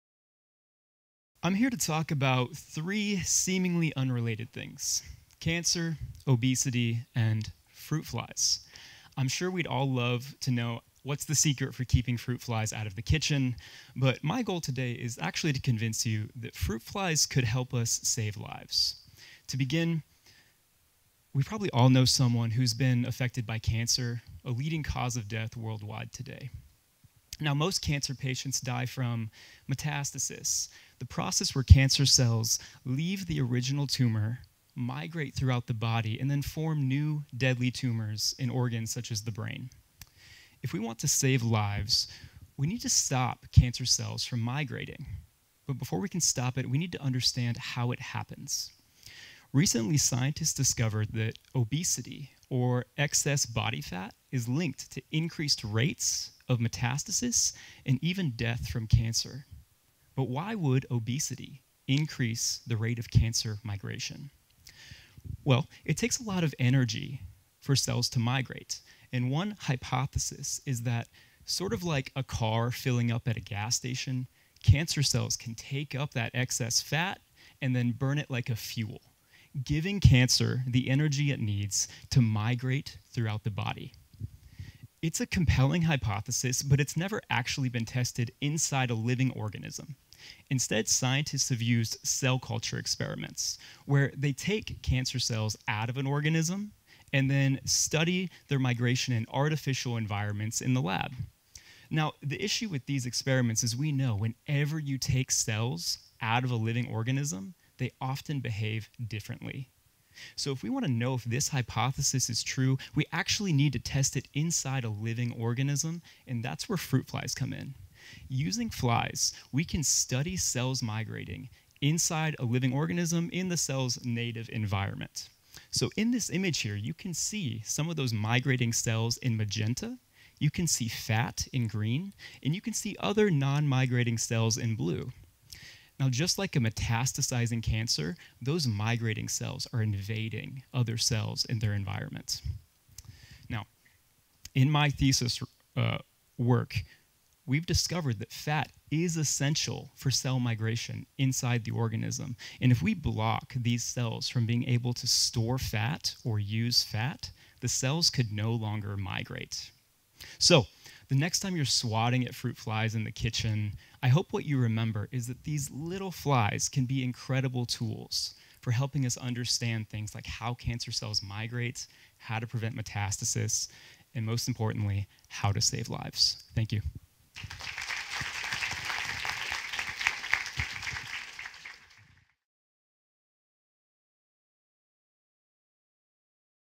3MT 2024 Winner Israel Wipf - Fueling the Fire: Fruit Flies and the Obesity-Cancer Connection
Three Minute Thesis Winner Israel Wipf:
Fueling the Fire: Fruit Flies and the Obesity-Cancer Connection

Israel Wipf doctoral candidate in Cell and Developmental Biology.

The 2024 Three Minute Thesis (3MT) is hosted by the University of Iowa Graduate College. See all 3MT '24 finalists: